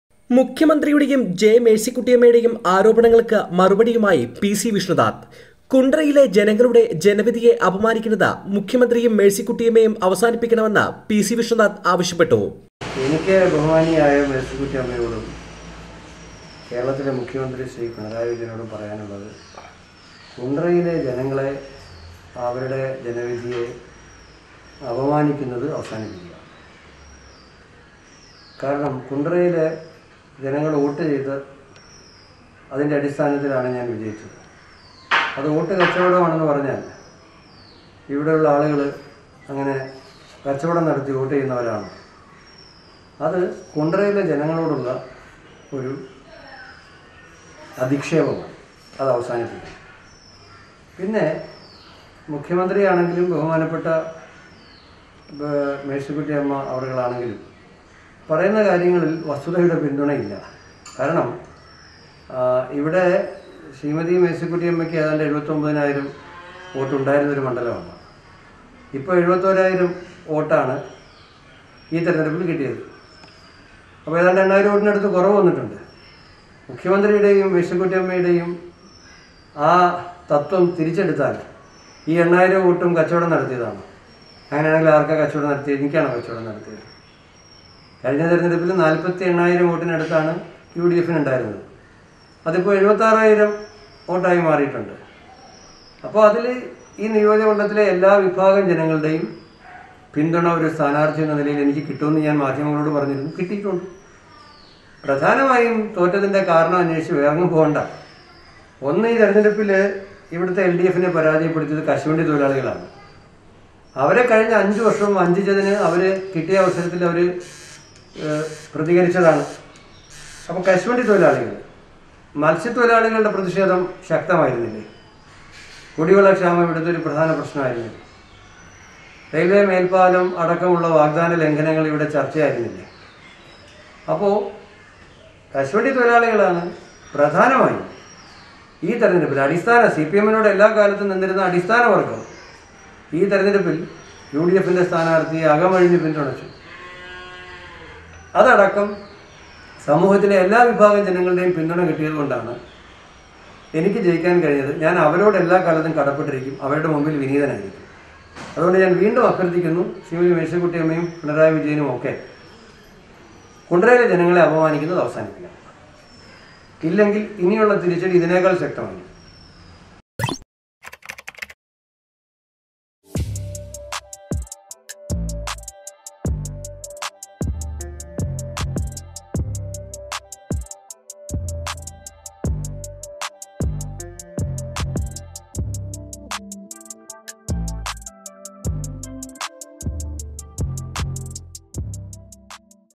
ബിജെപി വോട്ട് മറിച്ചോ? ആരോപണങ്ങൾക്ക് വിഷ്ണുനാഥിൻ്റെ മറുപടി
മുഖ്യമന്ത്രിയുടെയും ജെ മേഴ്‌സിക്കുട്ടിയമ്മയുടെയും ആരോപണങ്ങൾക്ക് മറുപടിയുമായി പി സി വിഷ്ണുനാഥ്.